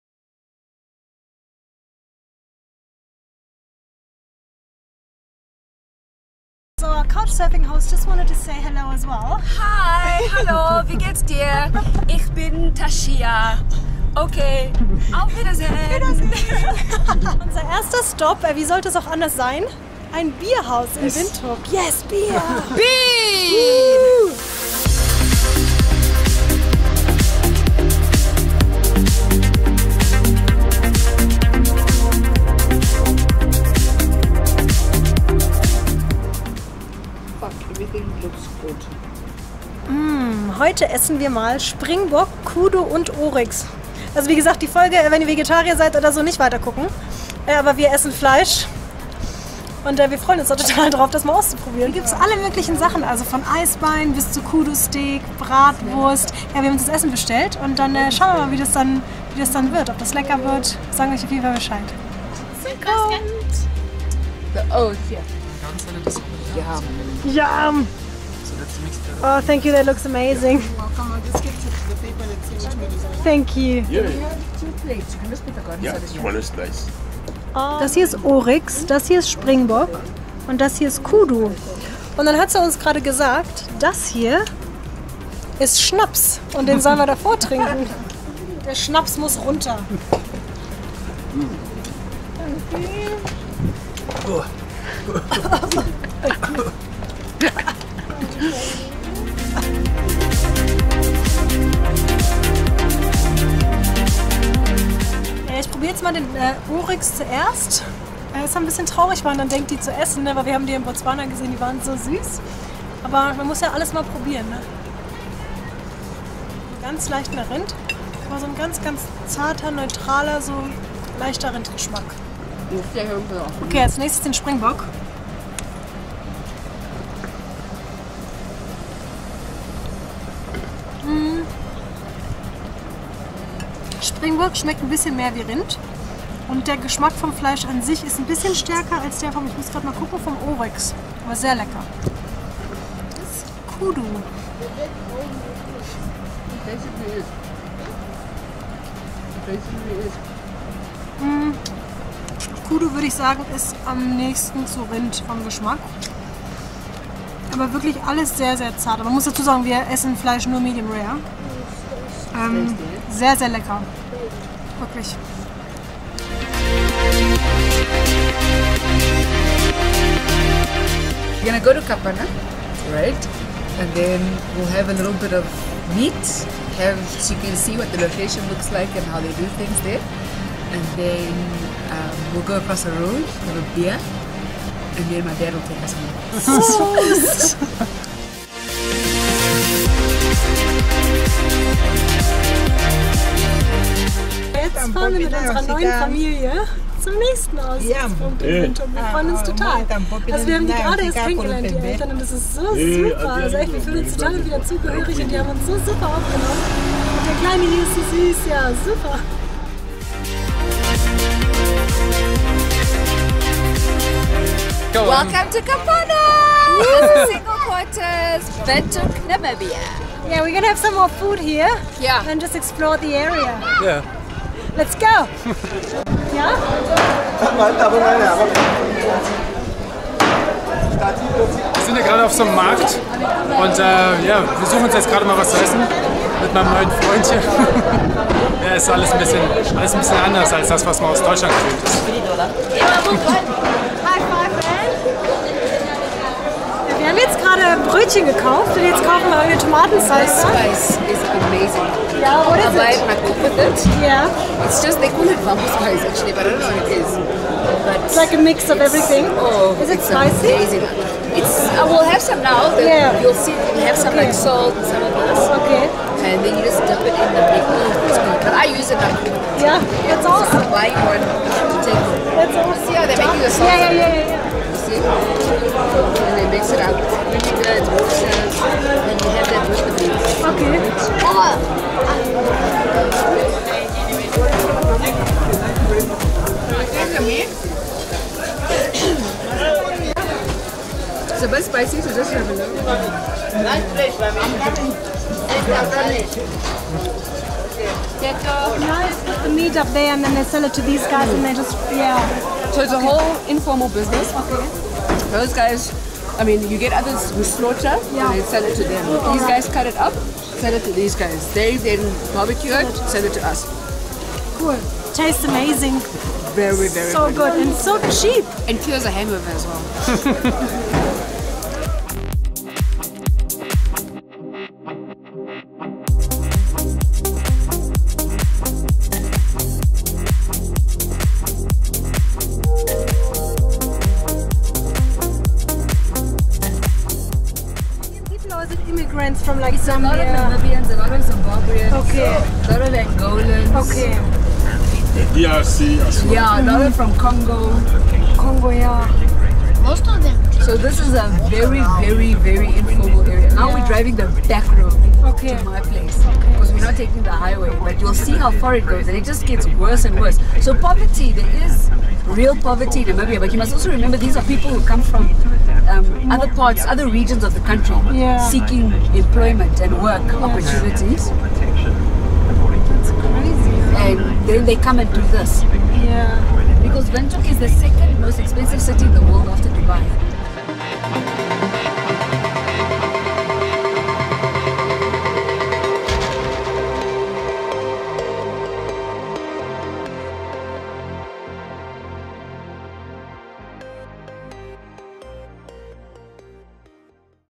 Authentisch Kapana & Fleisch essen in Windhoek, Namibia • Weltreise Vlog #012
Ab nach Windhoek! Auf der Suche nach authentischen Gerichten, wie es die Leute hier in Namibia gerne essen.

Zuerst fuhren wir zu Joe's Beerhouse, in dem wir eine Art Safari-Allerei aßen. Kudu, Oryx und Springbok. Danach ging es in die Informal Area Windhoeks, zum Kapana Essen. Diese Grillstände werden in den ärmeren Gegenden der Stadt betrieben und normalerweise verirren sich keine Bleichgesichter wir wir in diese Gegend.

Alles geht etwas rustikaler zu, vom Zerteilen bis zum Grillen wird alles direkt vor Ort gemacht. Serviert wird dazu frittiertes Brot und Tomatensalsa. Einfach, aber lecker!

Alle Infos zu unserer Selbstfahrer Safari in Namibia findest du auf unserem Blog:

Die Kameras mit denen wir filmen, findest Du in unserer Elektronik Packliste.